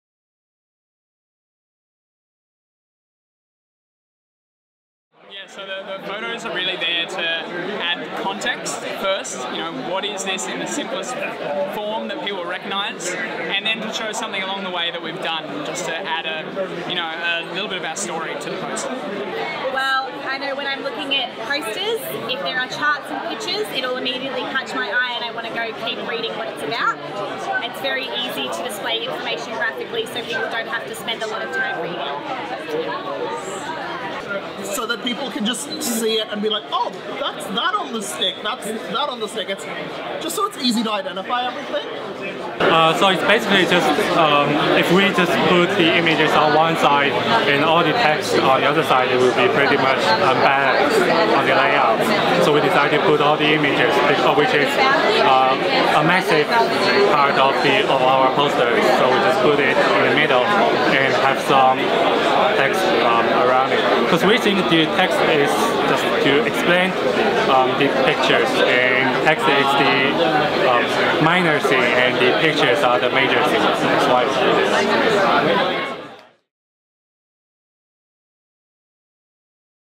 Designing a poster - using charts, images & diagrams
In this short video University of Melbourne students provide tips on how they use charts, diagrams and images in their posters. Want to hear more? Check out the other videos in this series: